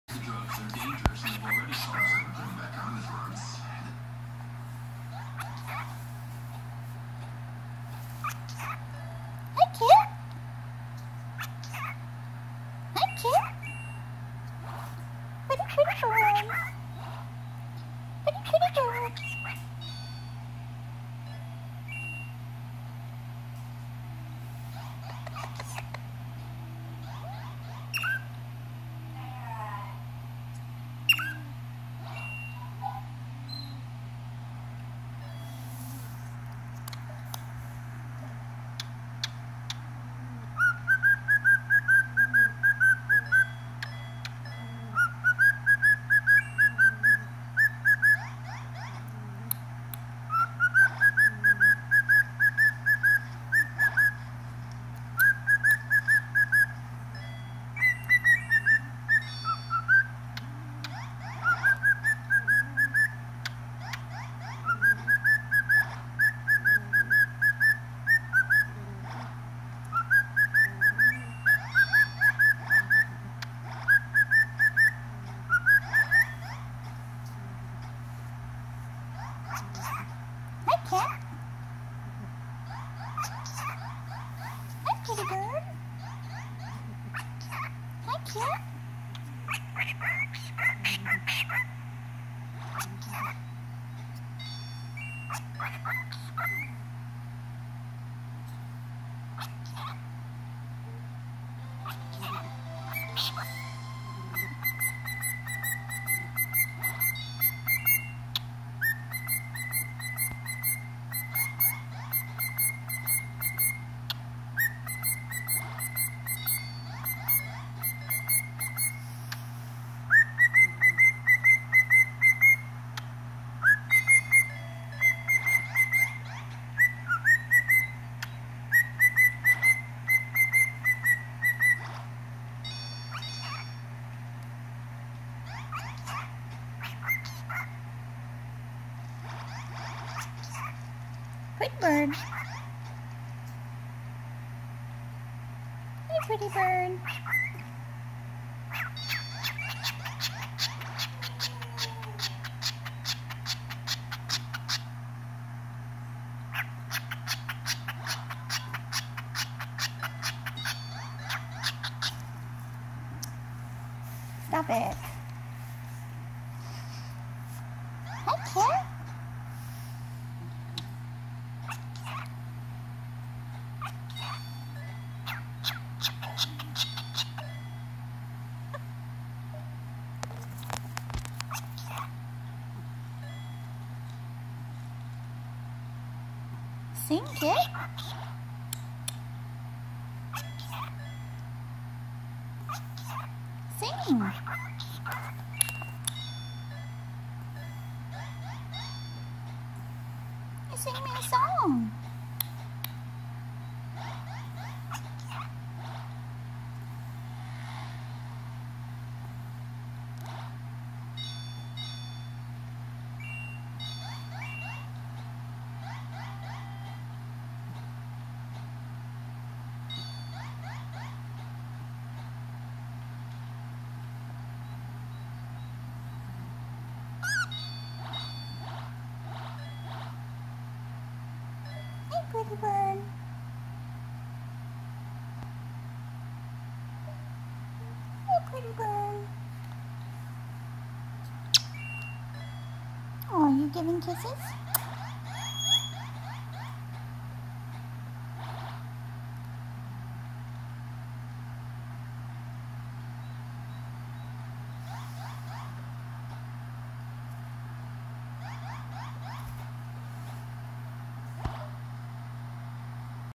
Cockatiel singing and talking to a cat.
Treats: fresh fruits. He loves broccoli.